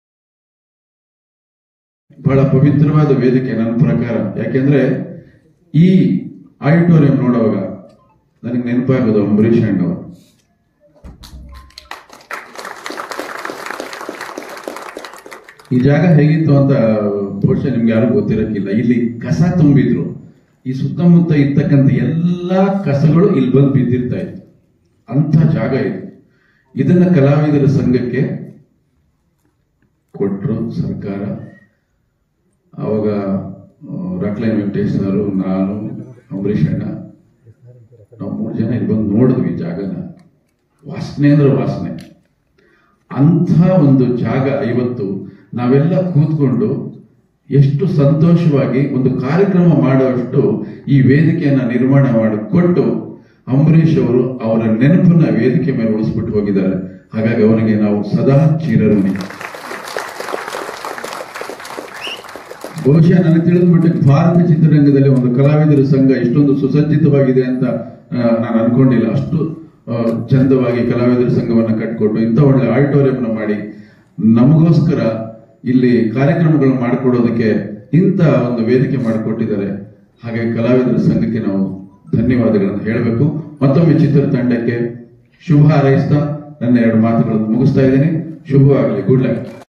ಅಂಬರೀಷಣ್ಣಗೆ ಜೈ
ಕಲಾವಿದರ ಭವನ ನಿರ್ಮಾಣದಲ್ಲಿ ಅಂಬರೀಷ್ ಕೊಡುಗೆ ಅನನ್ಯ

ಕಸ ತುಂಬಿದ್ದ ಜಾಗವನ್ನು ಅಂಬರೀಷ್ ಅಣ್ಣಾ ,ನಾನು ರಾಕ್ ಲೇನ್ ವೆಂಕಟೇಶ್ ನೋಡಿದಾಗ ಇಲ್ಲಿ ಏನೂ ಮಾಡಲು ಸಾದ್ಯವಾಗದು ಅನ್ನುವ ಹಾಗಿತ್ತು. ಆ ಜಾಗದಲ್ಲಿ ಸುಂದತ ಕಲಾವಿದರ ಭವನ ನಿರ್ಮಾಣ ಮಾಡಿದ್ದಾರೆ ಅಂಬರೀಷ್ ಅಣ್ಣ ಅವರ ನೆನಪು ಬಿಟ್ಟು ಹೋಗಿದ್ದಾರೆ